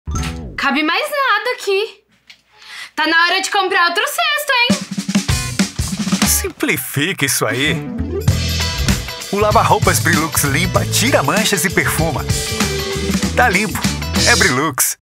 Simplifica isso aí com o Lava Roupas Brilux!
Tá difícil se livrar das montanhas de roupas sujas? Simplifica isso aí! O lava roupas Brilux é tripla ação: limpa, tira manchas e deixa aquele perfume que você ama.